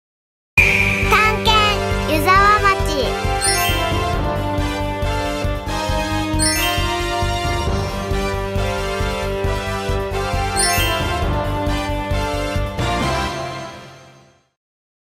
湯沢町🔎探県プロジェクト【PR動画：名物・魅力・見どころ編】
湯沢町には名物・魅力・見どころがたくさん！
みなさん、全部知っていますか？

①大源太キャニオン
②湯沢高原
③苗場の紅葉

💻探県サイト
探県サイトでは、いままで訪れた市町村情報を随時更新中！
ぜひ、こちらもチェックしてください🎵

📱探県公式Instagram
Instagramでは、探県のオフショットなども公開中！
こちらも、ぜひぜひチェック＆フォローをお願いいたします✨